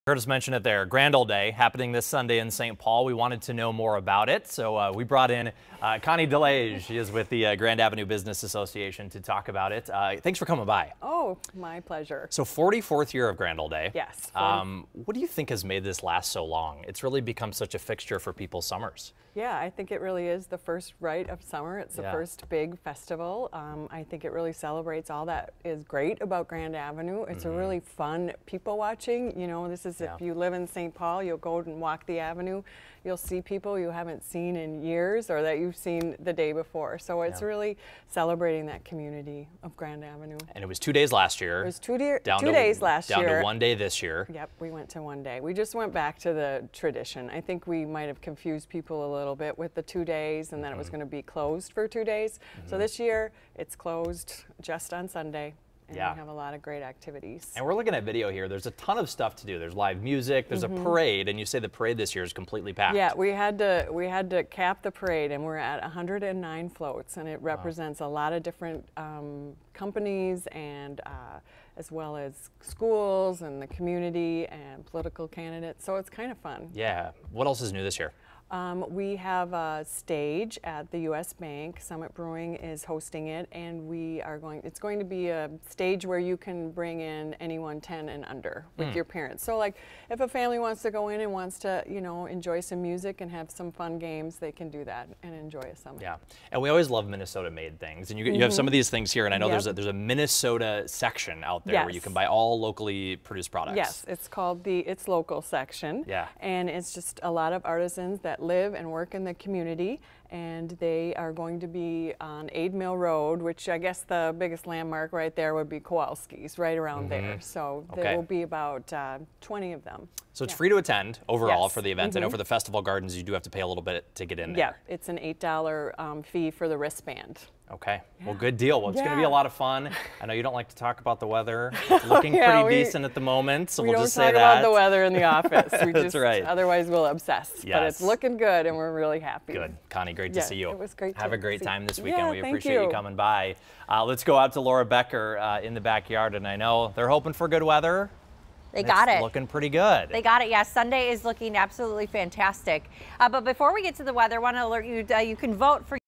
Grand Old Day on Sunday
Grand Ole Day is happening this Sunday is Saint Paul.